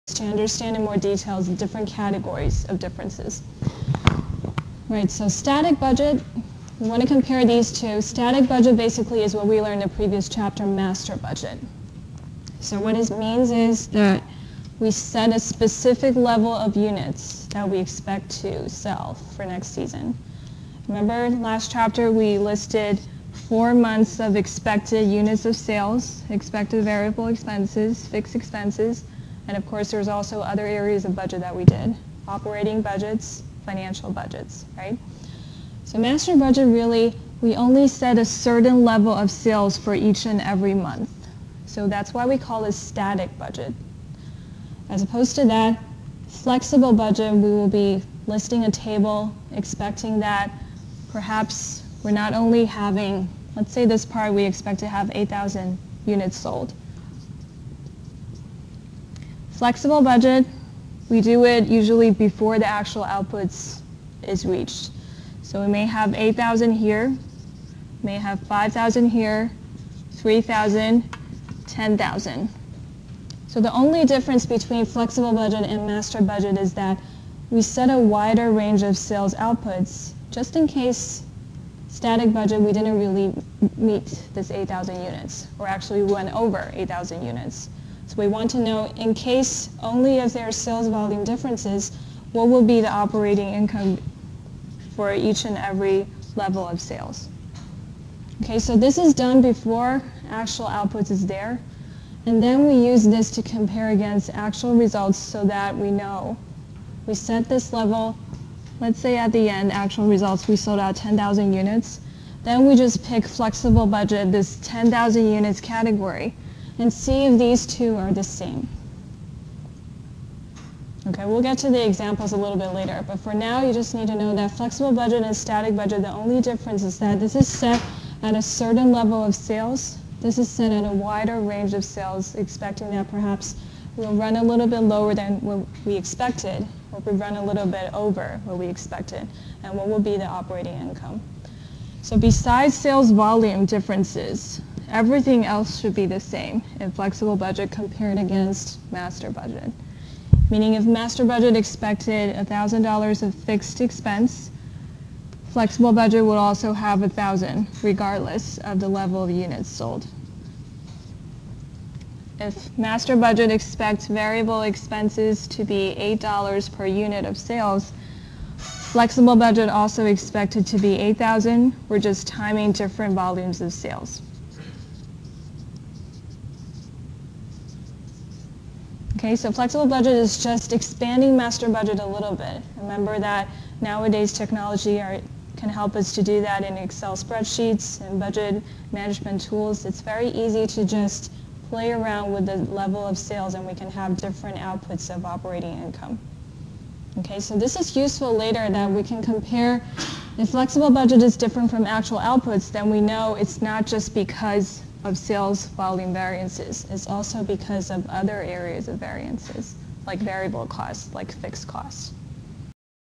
Static vs Flexible Budgets - Managerial Accounting
Description:

After giving an overview of the learning objectives to be discussed in the new topic (flexible budgets and standard costs), the Professor goes on to fully define flexible budgets (and also gives a visual). Variances are also discussed - the Professor goes over variance analyses (using a flow chart, which shows how they relate to each other). The variances covered include price variances, efficiency (quantity) variances, direct labor variances, and direct material variances. Several exercises are completed pertaining to every topic covered (solutions and explanations included).